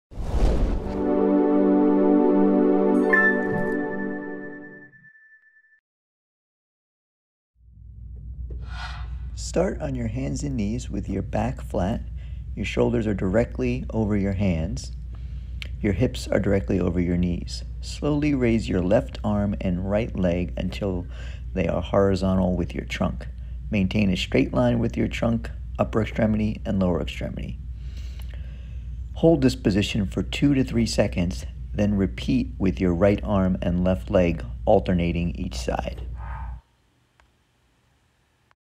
Bird Dog
The bird dog exercise strengthens the smaller low back muscles.  Perform 10 to 20 repetitions on each side.  BSR Physical Therapy - Barnegat and Manahawkin, NJ.